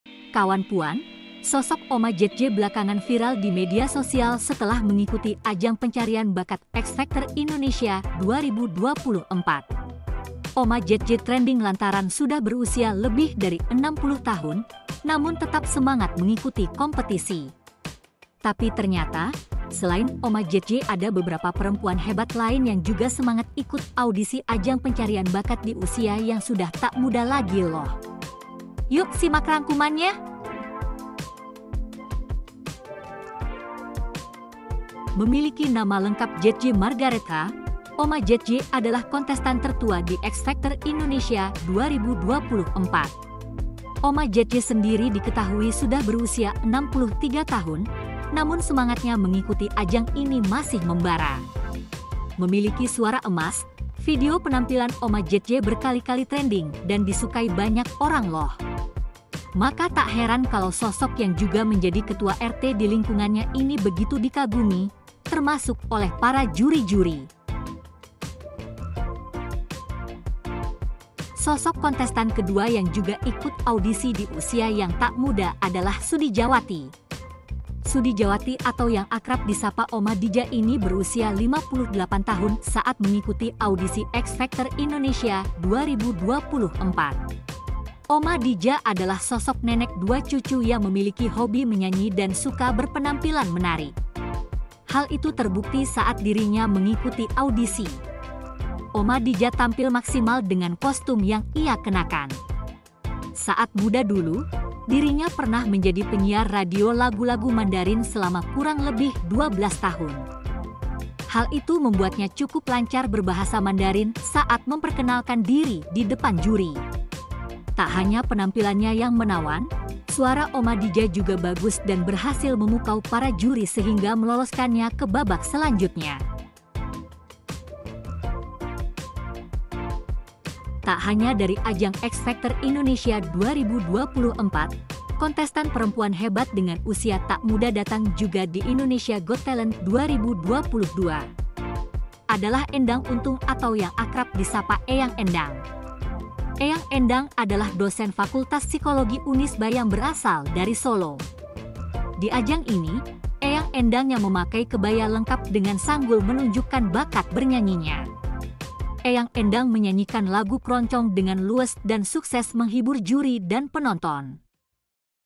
Ada Oma Jetje X Factor Indonesia,Ini 3 Kontestan Perempuan Tertua di Ajang Pencarian Bakat Indonesia
Oma Jetje trending lantaran sudah berusia lebih dari 60 tahun, namun tetap semangat mengikuti kompetisi.